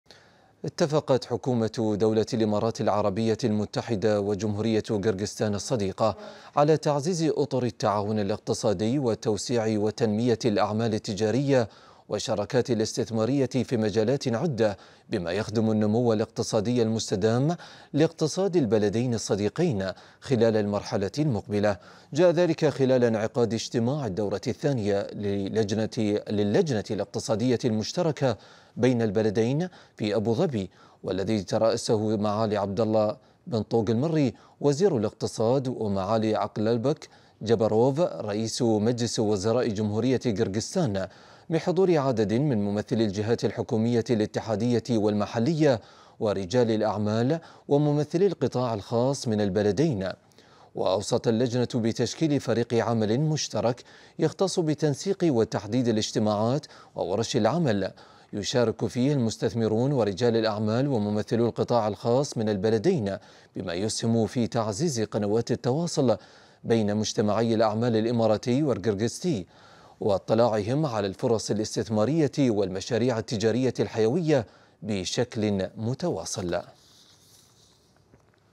الإمارات وقيرغيزستان تتفقان على زيادة الاستثمارات المتبادلة خلال انعقاد اللجنة الاقتصادية المشتركة
أخبار الدار | الإمارات وقيرغيزستان تتفقان على زيادة الاستثمارات المتبادلة خلال انعقاد اللجنة الاقتصادية المشتركة